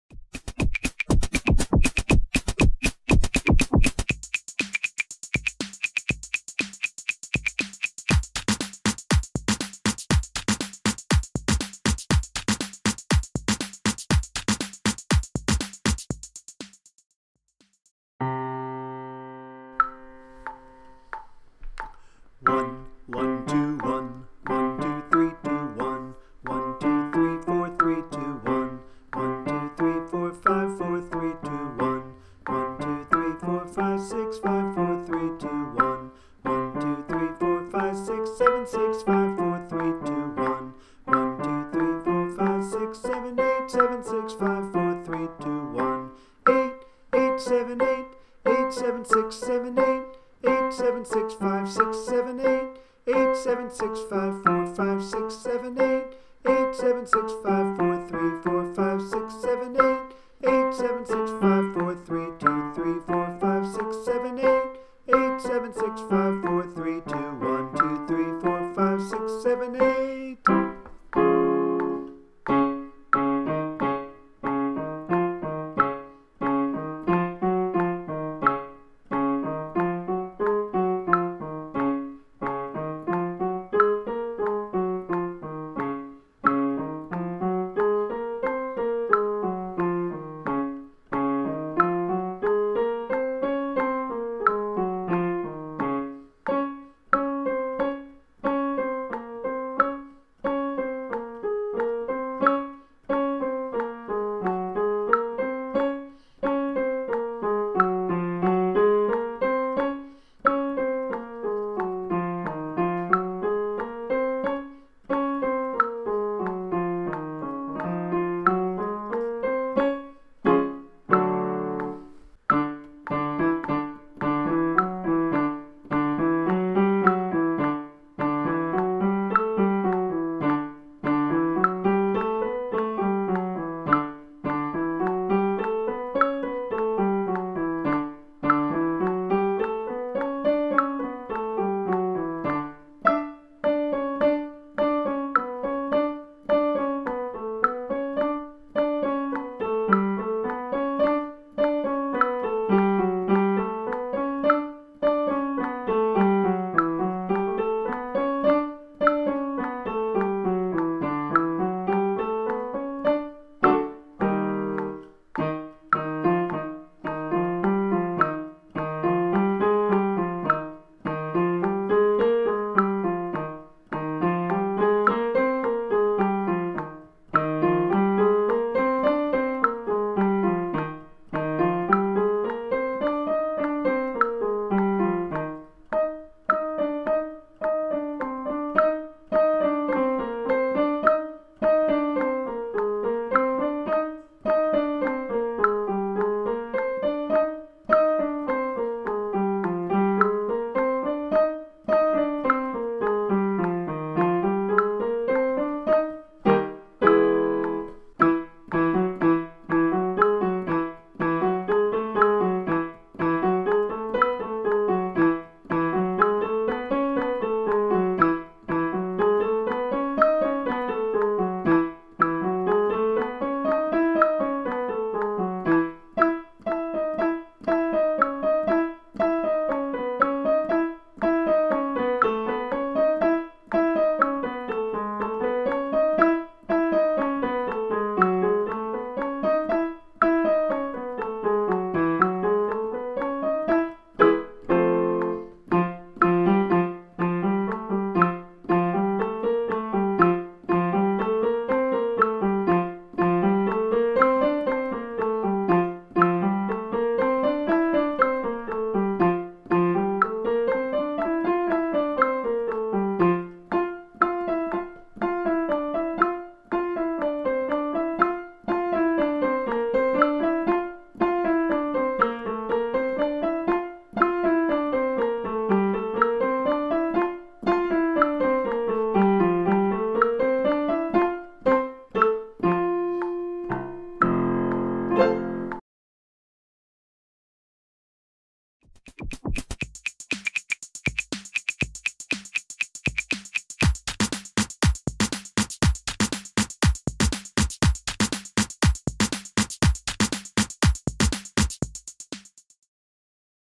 Choir Warm ups - 1 121 12321 etc.
Please practice this fun warm-up using the numbers.  It is ascending and descending.

Intro: 0:00
Beginning: 0:17
A little Faster: 2:00
Even Faster: 3:26